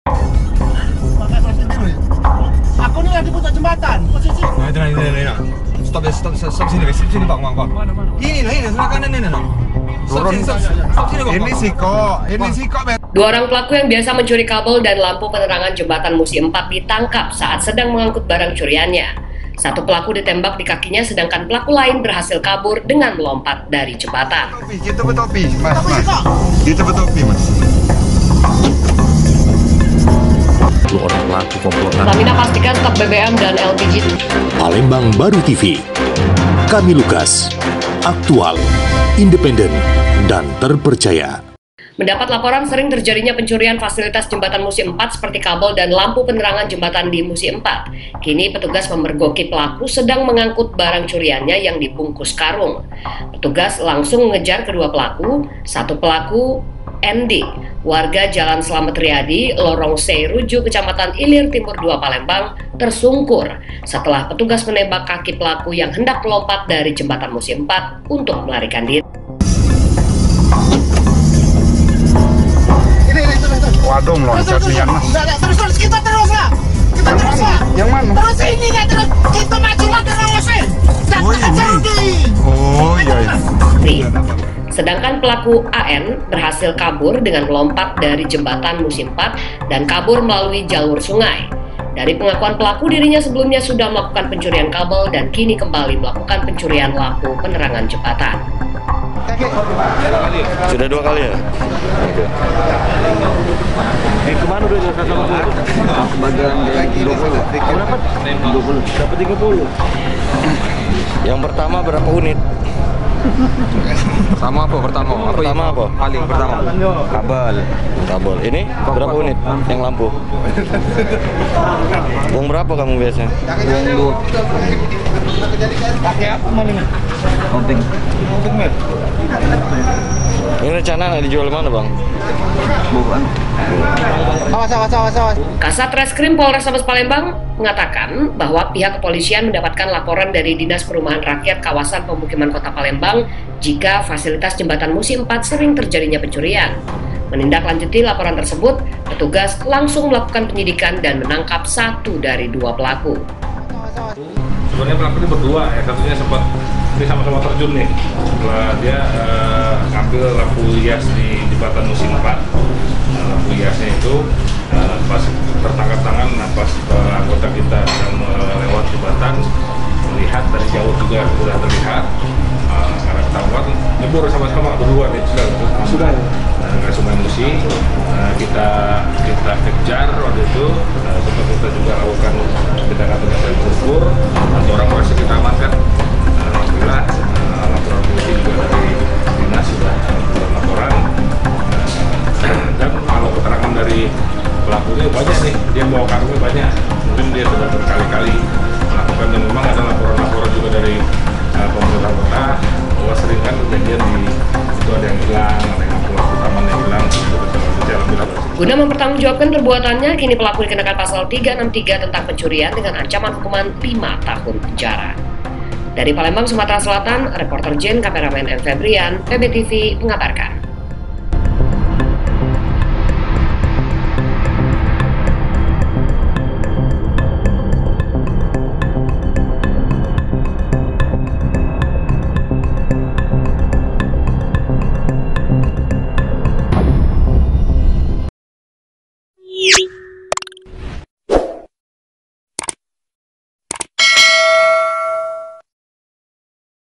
Kepergok Polisi Curi Lampu Jembatan Musi IV, Satu Dari Dua Pelaku Nekat Lompat Ke Sungai
Liputan : M Febrian
Reporter : Jane